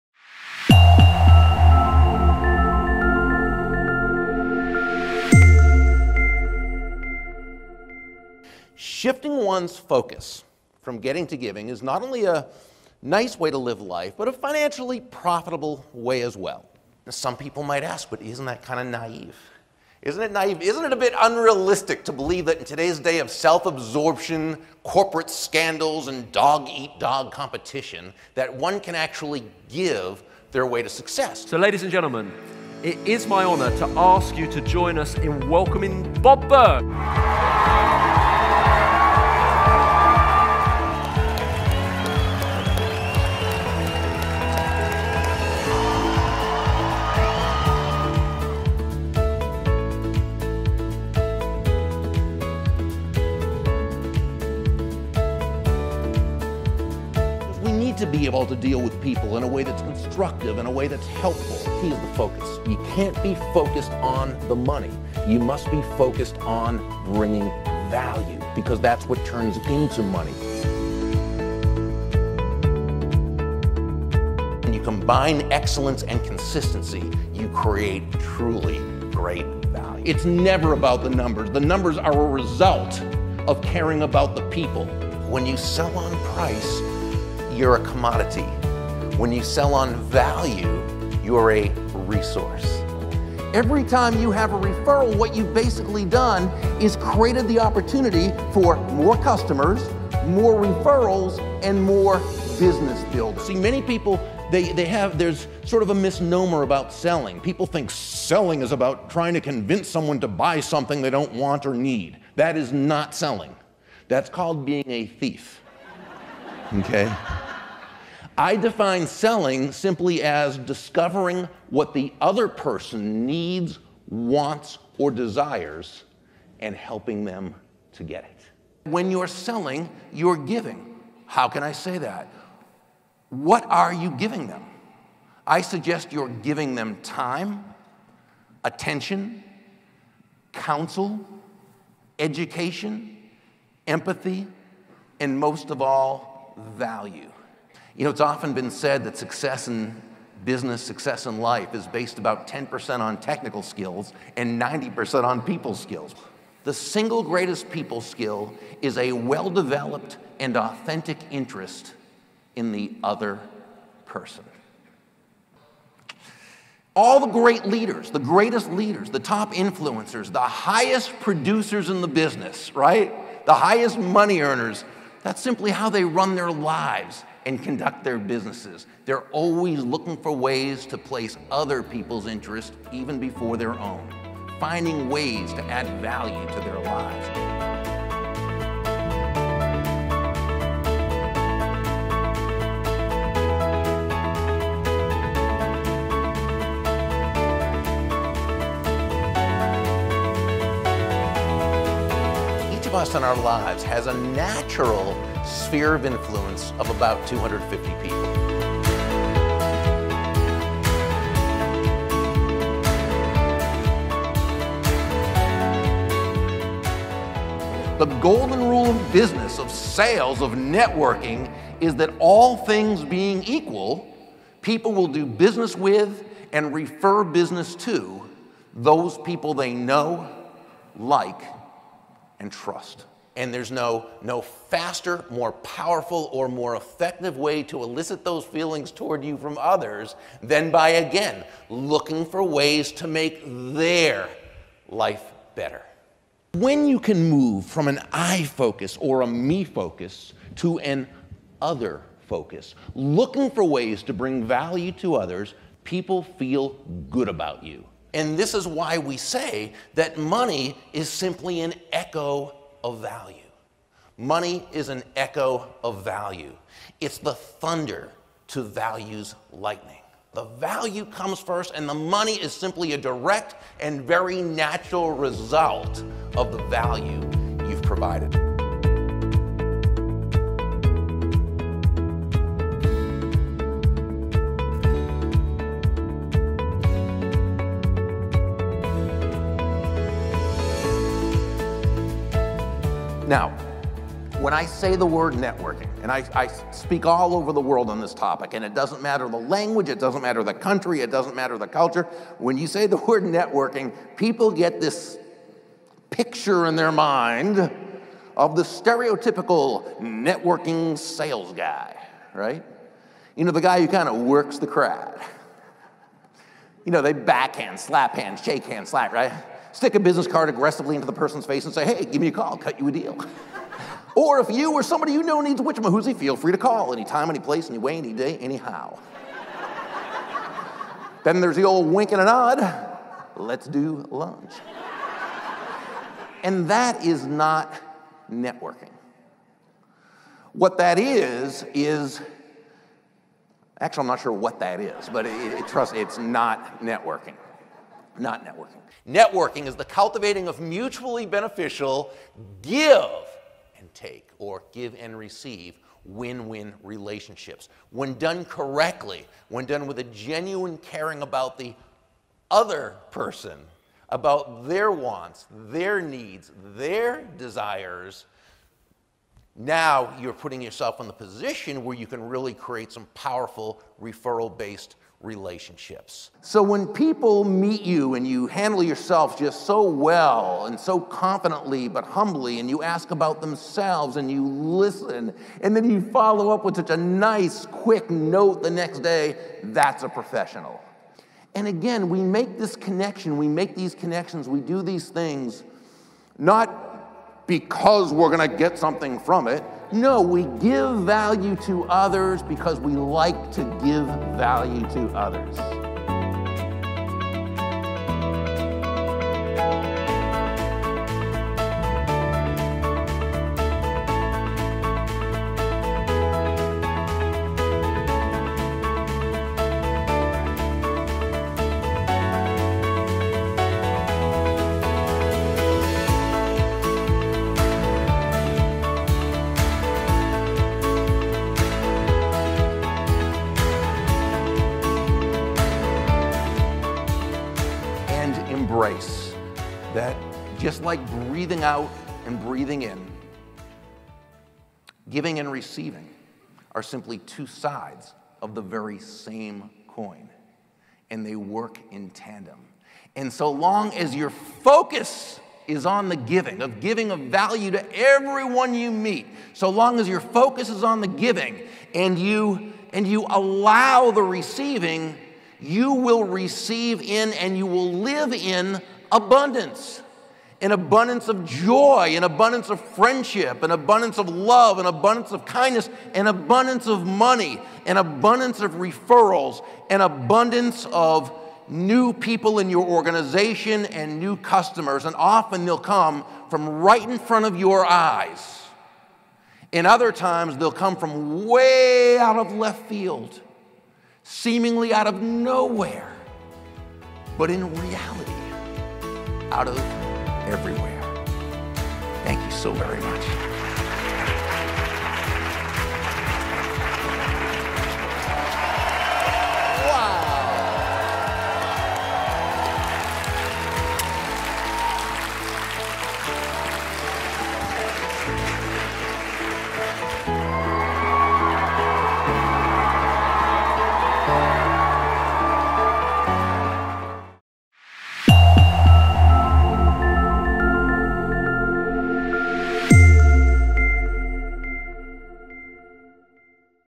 Bob Burg - Speaking Preview
Bob Burg shares information on topics vital to the success of today’s businessperson. He speaks for corporations and associations internationally, including fortune 500 companies, franchises, and numerous direct sales organizations.

Bob regularly addresses audiences ranging in size from 50 to 16,000 — sharing the platform with notables including today’s top thought leaders, broadcast personalities, Olympic athletes and political leaders including a former United States President.

Although for years he was best known for his book Endless Referrals, over the past few years it’s his business parable, The Go-Giver (coauthored with John David Mann) that has captured the imagination of his readers.